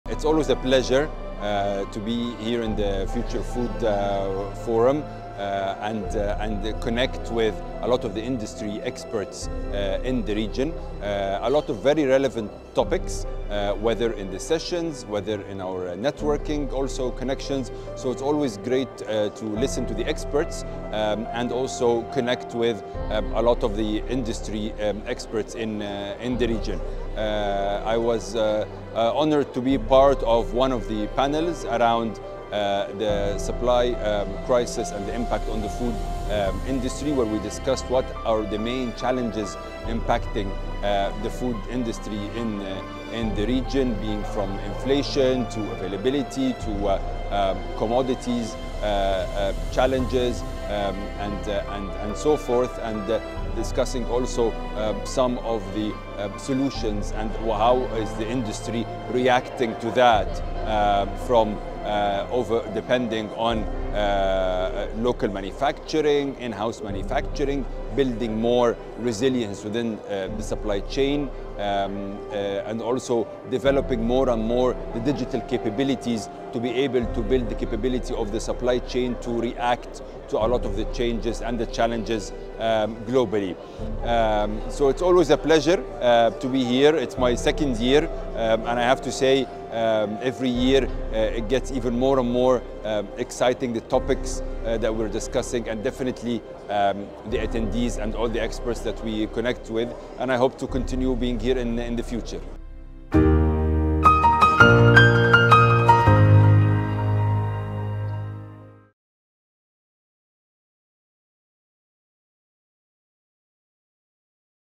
Future Food Forum 2022 | Sherif Hafez | Day 2 Interview
Among its crucial topics, the Future Food Forum 2022 included a panel discussion on managing crises within the F&B Supply Chain.

Sherif Hafez, Supply Chain Director - MEA, Mars Wrigley highlights the discussions on the key challenges in the F&B industry as a part of the Future Food Forum 2022

Follow us for more insightful talks and updates on the Future Food Forum 2022.